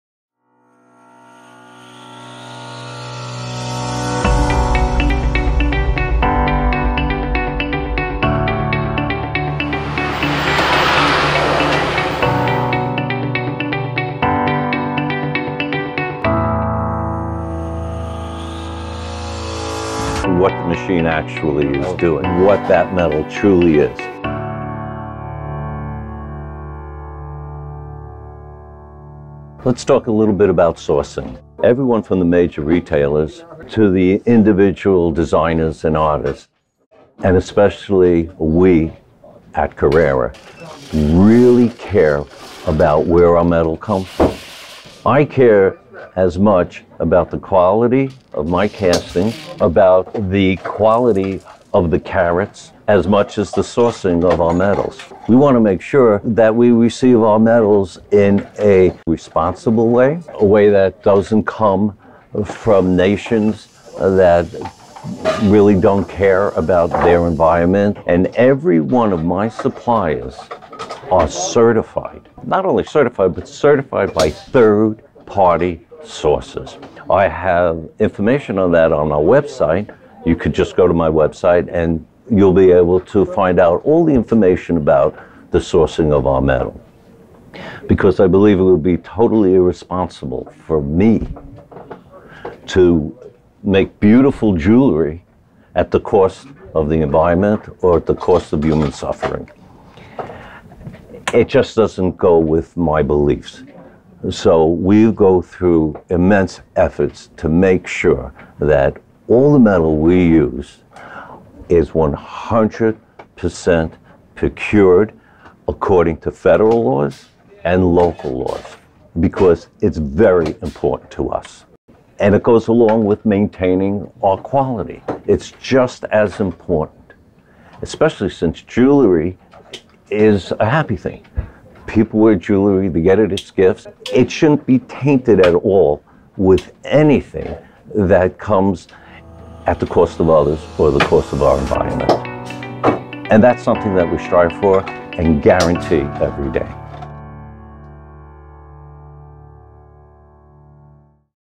Metallurgy Episode 4 Sustainability
In an effort to clarify the important topic of “Sustainable Sourcing”, Carrera’s metal expert Eric Schwartz walks us through the process. Today's customers want to know where their metals come from, how was it procured and how were the people treated that produced it.
See how all of our metals have third party verification that they were procured in an ethical and legal manner.

Not only are we passionate about the source of our metals, but we guarantee it.